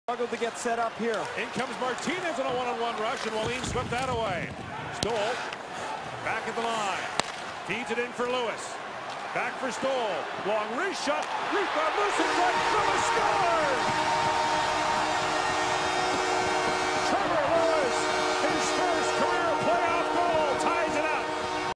Trevor Lewis First Career Playoff Goal (4/25/11) [HD]
An important goal made late, but unfortunately for the Kings, the Sharks win in OT after killing a 5 minute penalty. Unfortunately, this was the last goal score by the Kings this year. Congrats to the San Jose Sharks